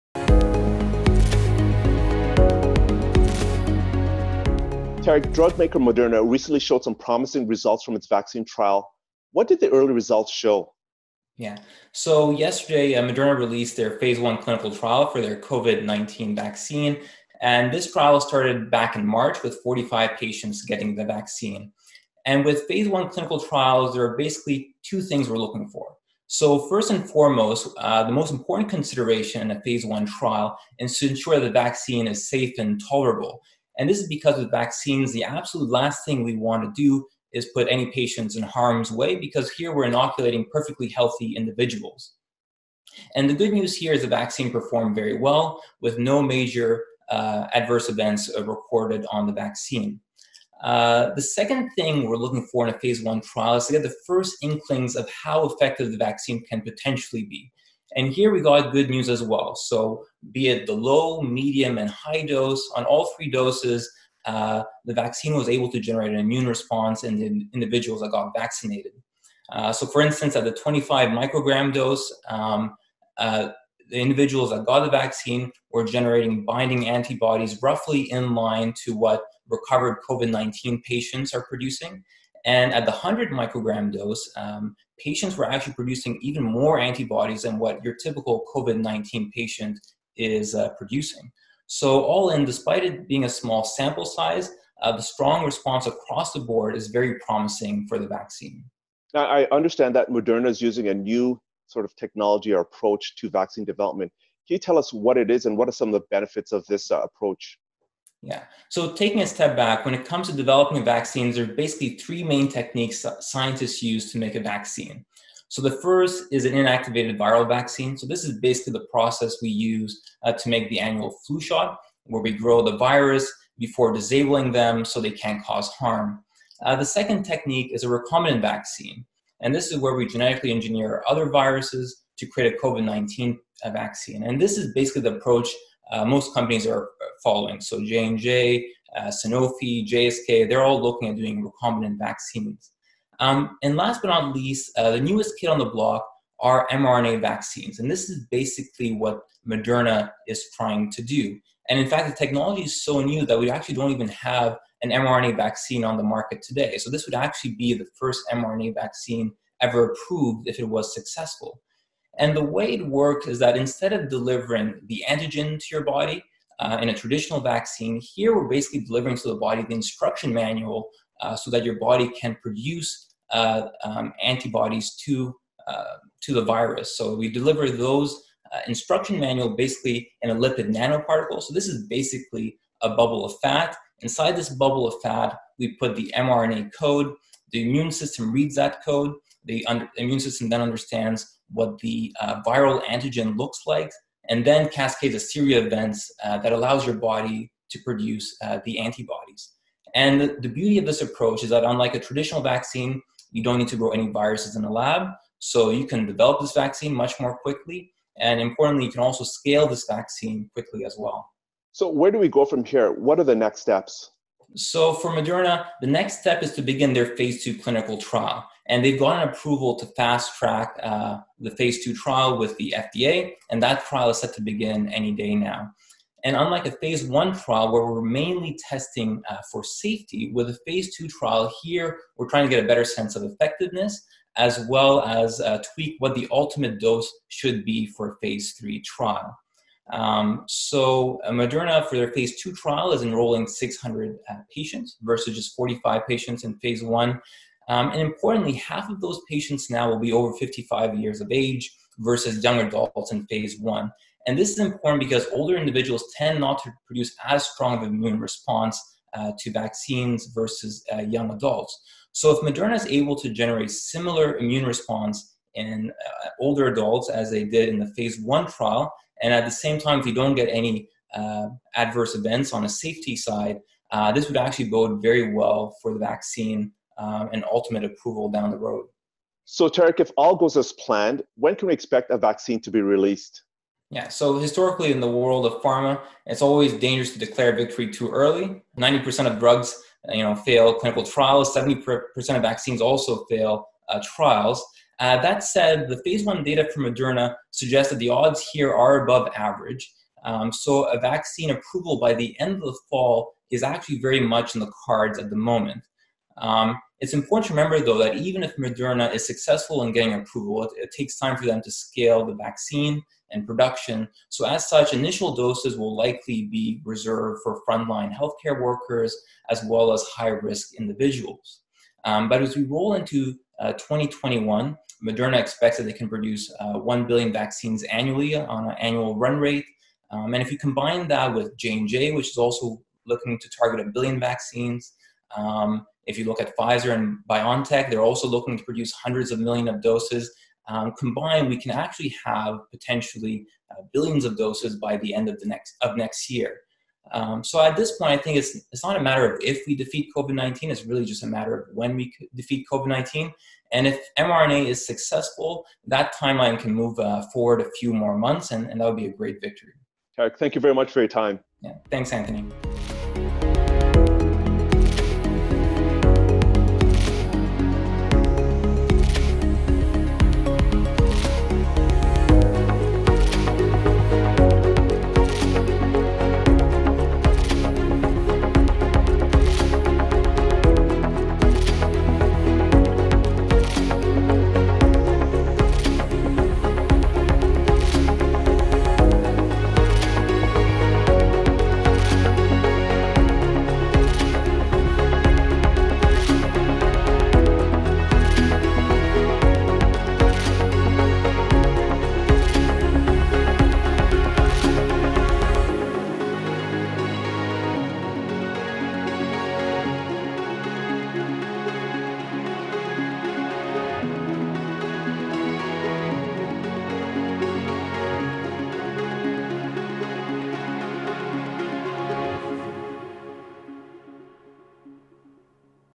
Moderna early trial results raise COVID-19 vaccine hopes
The race to find a COVID-19 vaccine is heating up. Anthony Okolie speaks with Tariq Aeta, Healthcare Analyst, TD Asset Management, about the positive early results from the Moderna coronavirus vaccine trial.